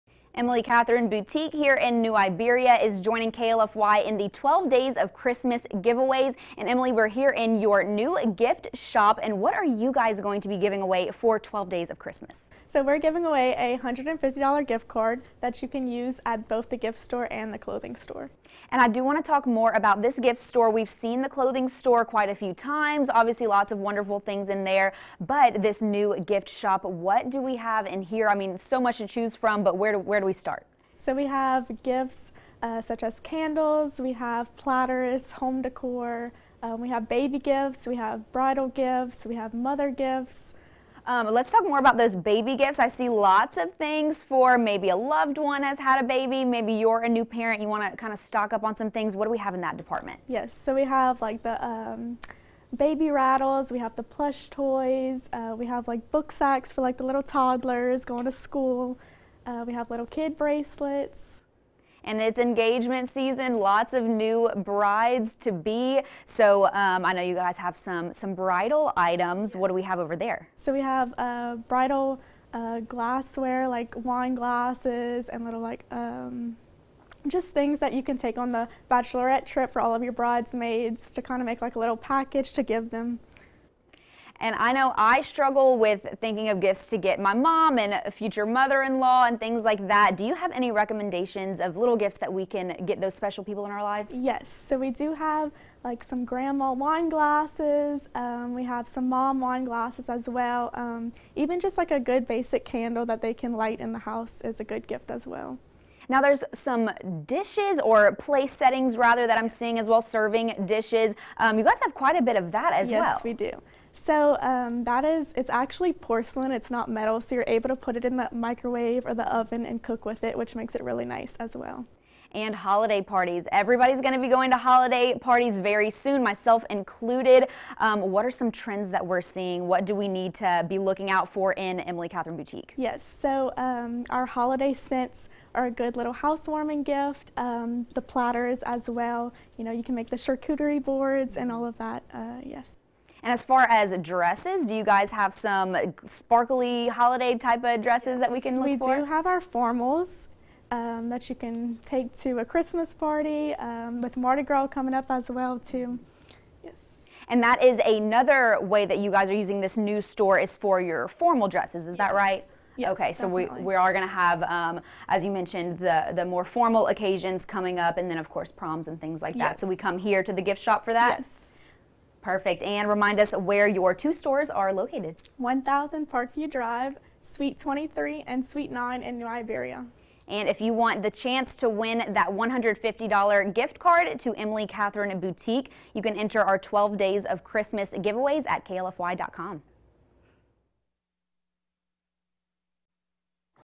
Acadiana Live: 12 Days of Christmas
Emily Katherine Boutique in New Iberia is giving away a gift card as part our our 12 Days of Christmas. Enter to win at KLFY.com.